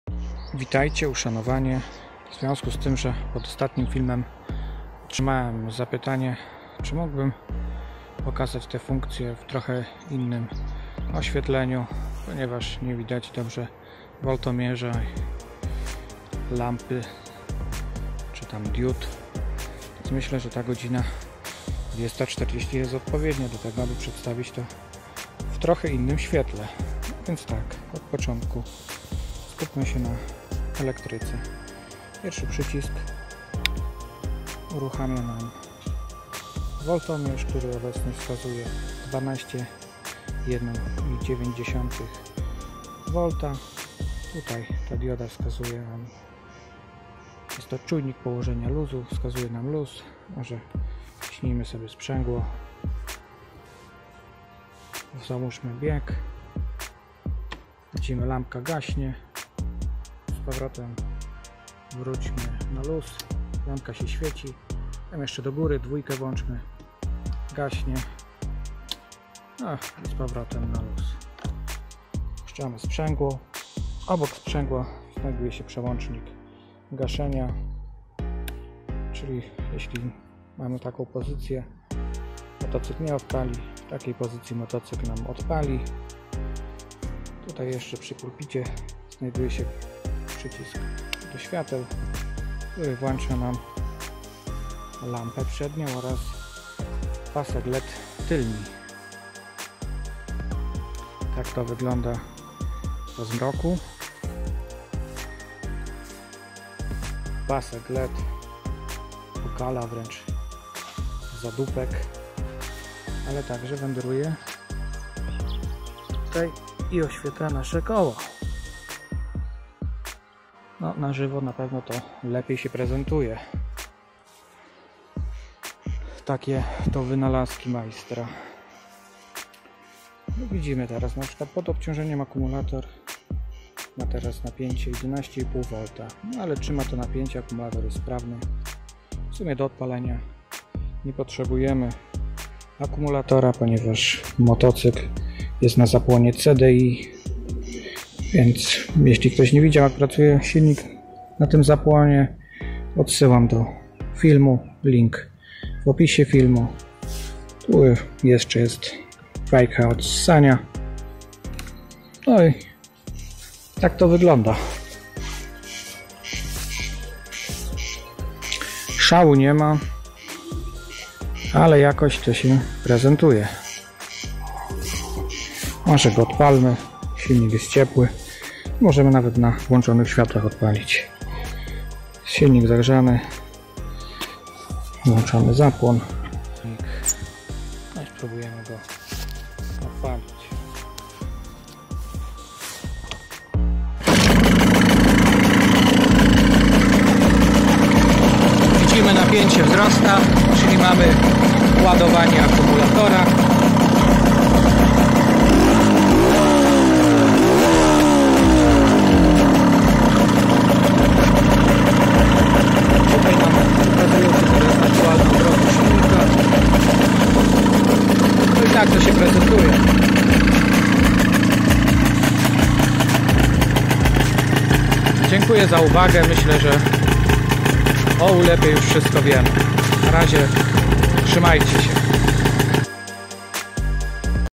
KTM SX 125 z silnikiem Mz
Film przedstawia ulepa czyli motocykl złożony z crossa ktm oraz silnika Mz Etz 150. W motocyklu wprowadzono pewne modyfikacje, zachęcam do oglądania. ;)